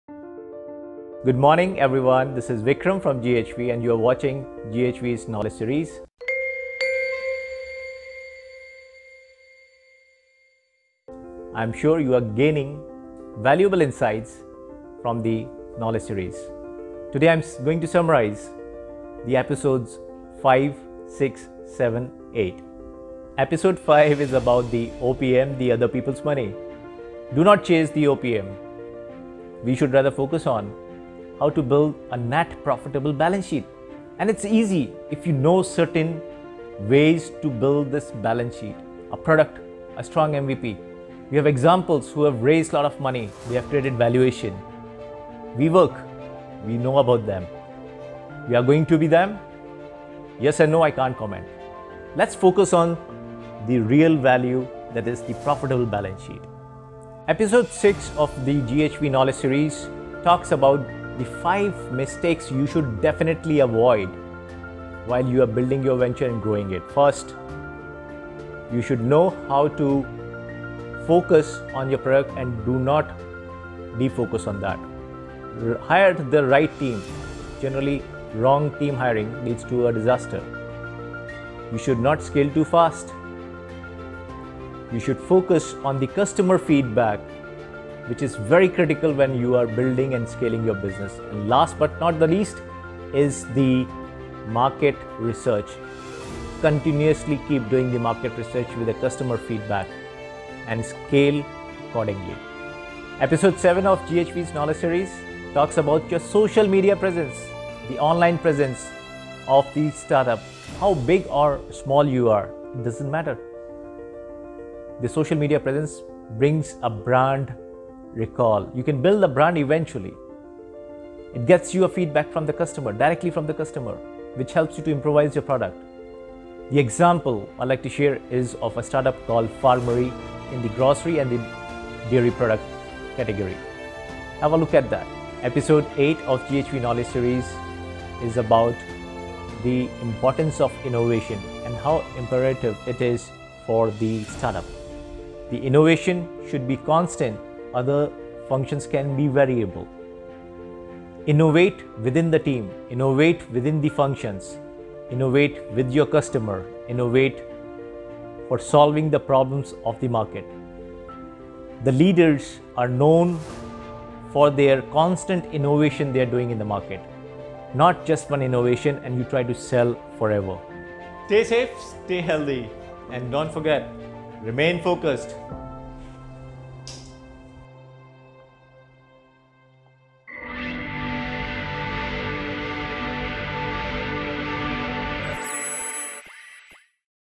A quick synopsis of the episodes 5, 6, 7, & 8 of the knowledge series
India has the second-largest ecosystem in the world, expected to witness YoY growth of 10-12%, with 30+ unicorns.

But the real questions arising are: Are they all successful? What if you are stuck and you are not aware about it? What if you scale to a certain point and then the needle stop moving? When is the right time to innovate? What are the mistakes to avoid while scaling?

Here is a quick recap of the episodes by Mr. Vikram Upadhyaya, to help you with the challenges you might encounter while building your business and how you can address them.

Episode 6: 5 Mistakes that can prove fatal for a startup

Episode 7: Why establishing a strong social presence is vital for a startup

Episode 8: Why it's imperative with startups to keep up with innovation

Check out this space to learn more!